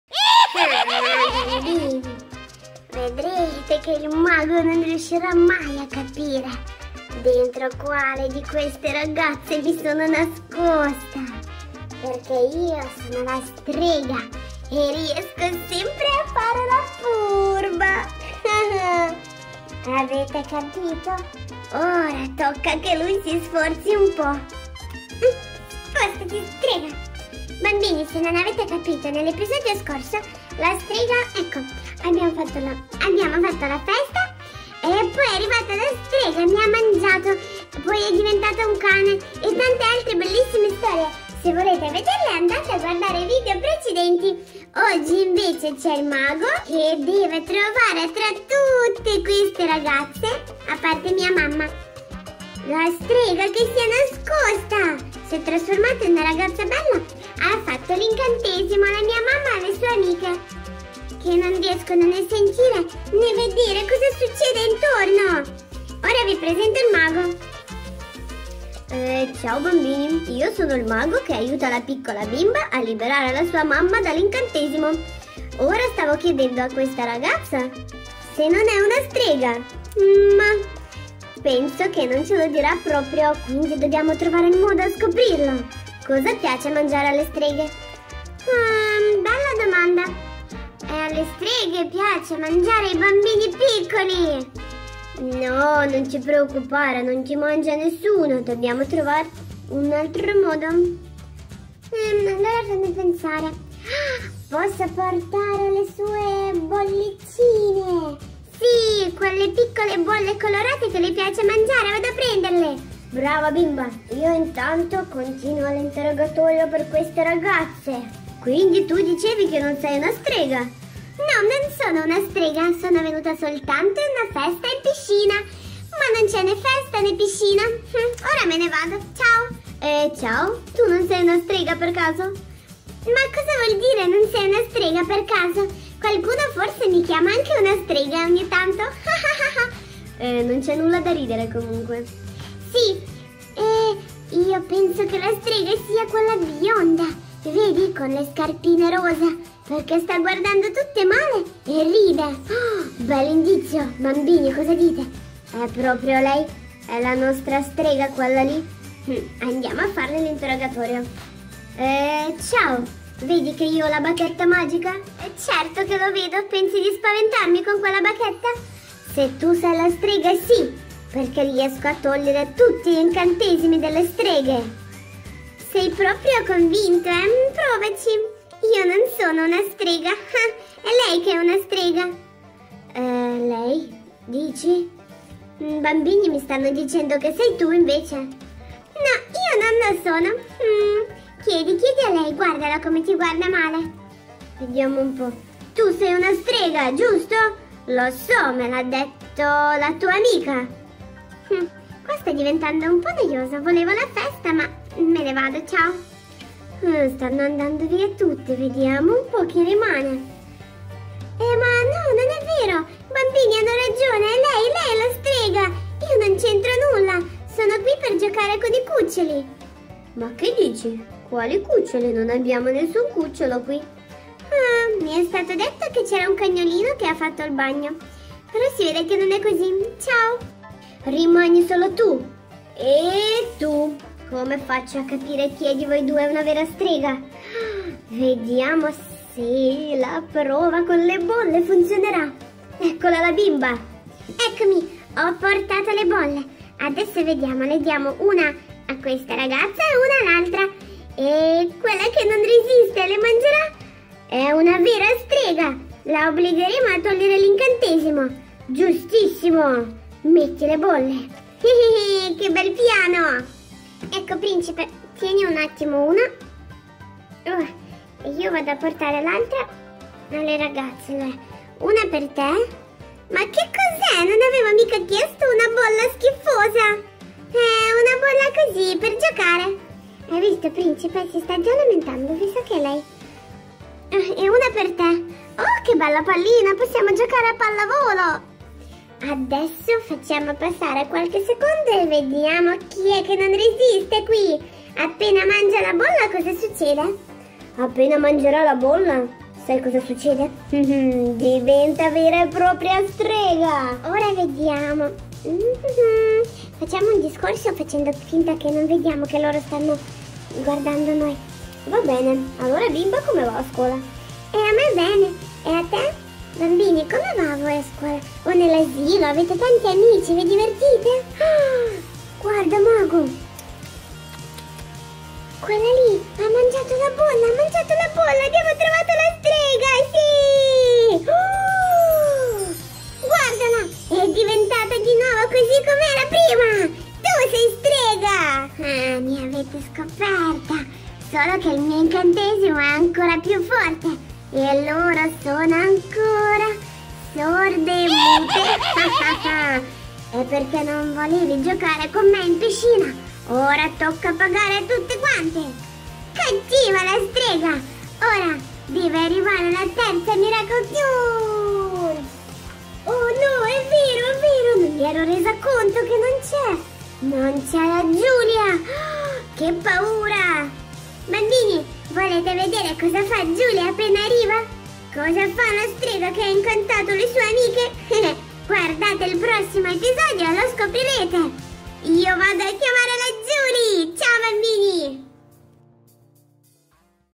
Parte 4: Strega si TRASFORMA in UNA BELLISSIMA BARBIE e INGANNA tutti 👧!
Strega inganna tutti!